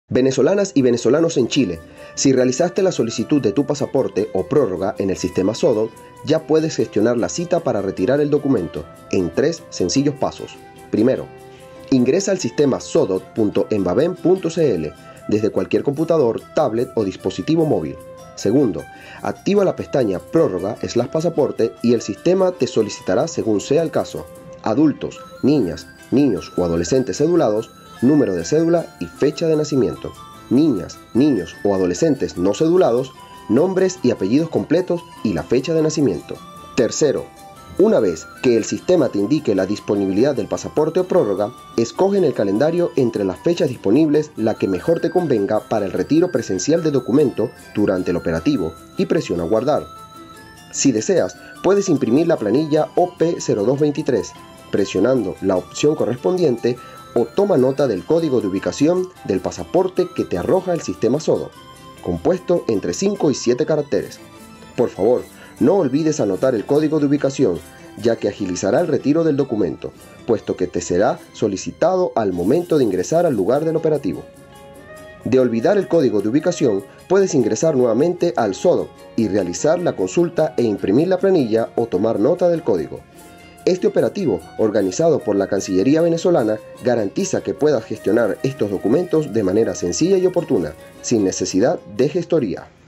Jornada de Entrega de Pasaportes en Chile 🇨🇱 Paso a paso
📹 EnVideo | Conoce los pasos a seguir para ser atendidos en la Jornada de Entrega de Pasaportes en Chile 🇨🇱, la cual se estará desarrollando del 13Feb al

iDile NO a la gestoría!